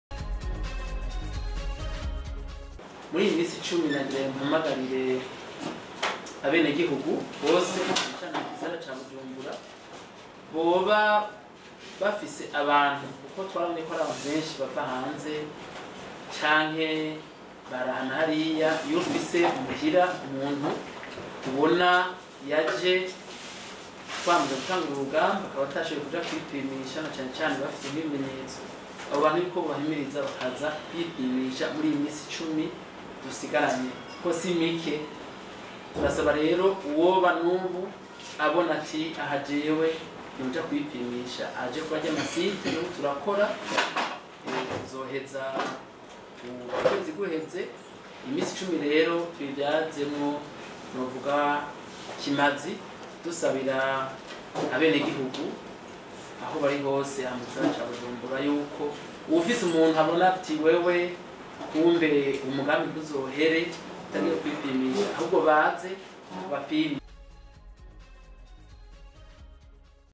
Burundi : Isekeza ry'ugupima Coronavirus ku buntu Coronavirus risigaje iminsi cumi
Burundi : Mu gihe hasigaye iminsi cumi kugira Isekeza ry'ugupima Coronavirus ku buntu, Umushikiranganji w'amagara y'abantu ahamagarira abantu bose bibonako ibimenyetso kwipimisha uwo mugera.